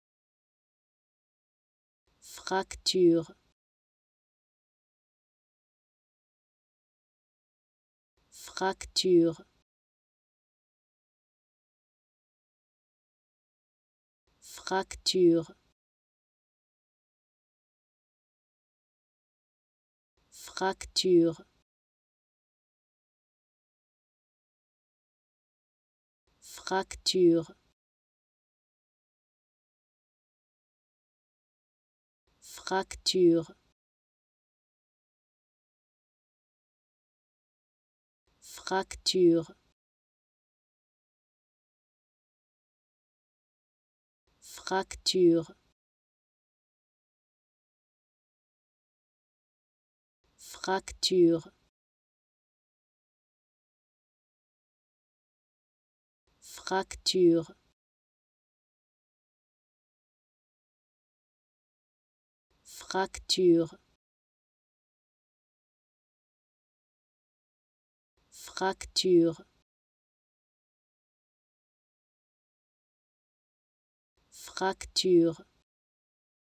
How to Pronounce ''Fracture'' Correctly in French
Learn how to say and properly pronounce ''Fracture'' in French with this free pronunciation tutorial. Apprenez à prononcer des mots en français grâce à nos tutoriels.